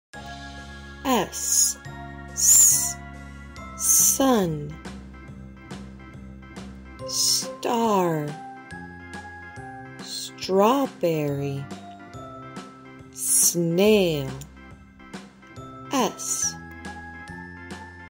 Letter Ss!
Pronunciation of English letter Ss along with word examples and music!